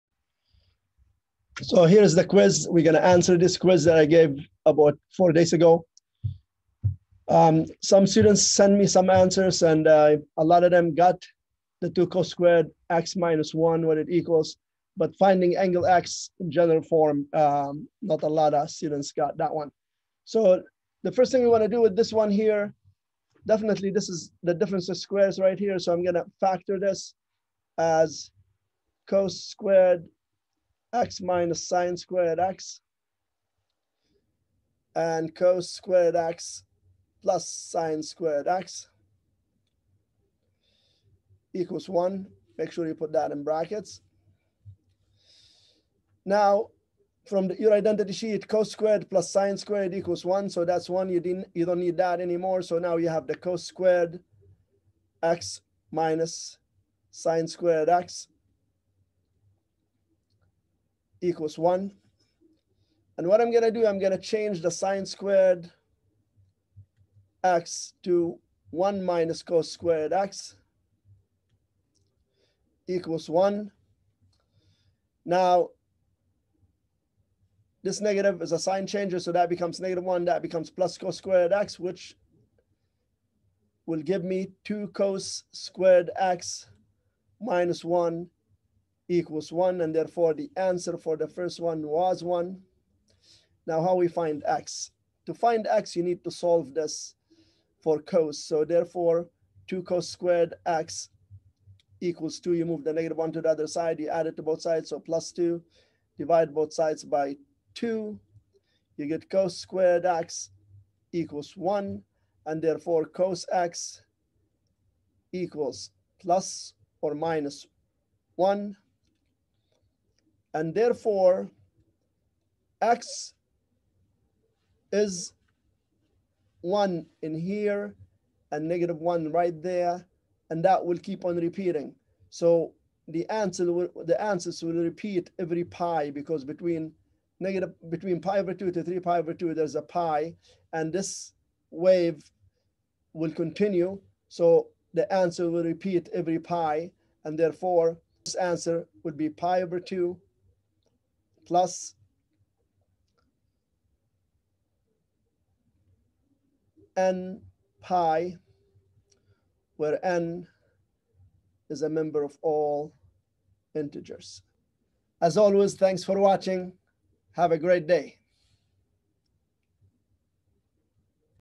Trigonometry Quiz.
Graphed the Sine instead of the Cosine. therefore, x = nπ, n∑I. Interpolated the angle from the Sine graph rather than the Cosine graph, The correct answer is nπ.
Pythagorean Identity.
The double angle identity.
Cross multiplying is the best Algebra tool that I advice all students to master.
Strong foundations in Math from middle school will elevate child's confidence in their education journey.
I live in Victoria BC, but through zoom and YouTube I am able to reach students all over the world and make a difference. Success in learning, success in life
Math is like a puzzle, and puzzles are fun to solve.